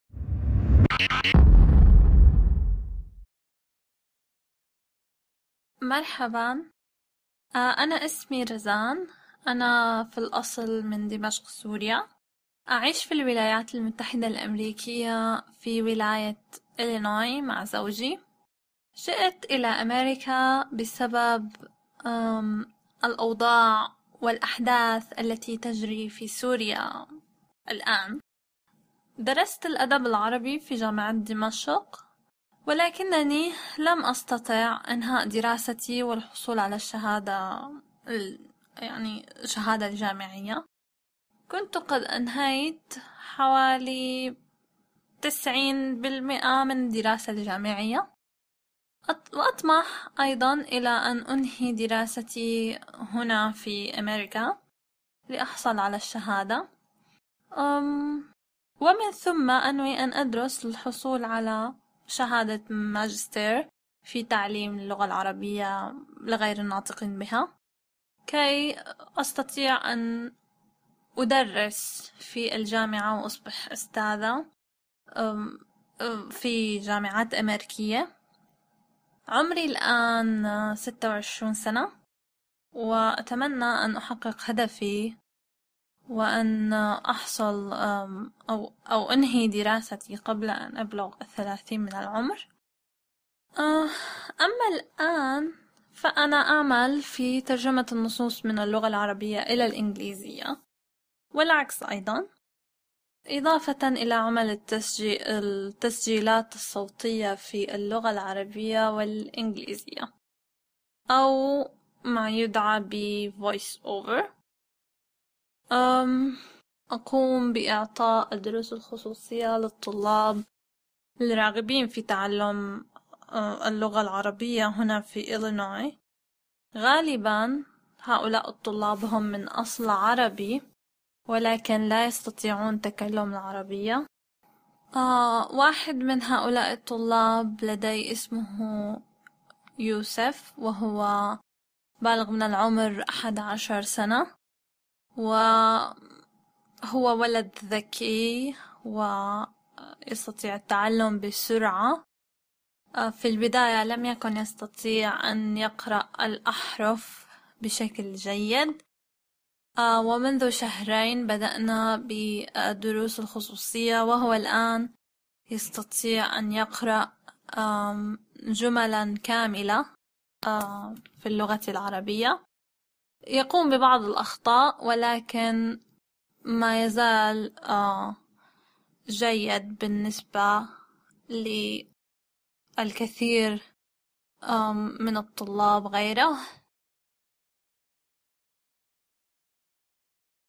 Arabic Voices 1.01 - Razanne's Introduction
Razanne (from Syria) introduces herself in Modern Standard Arabic.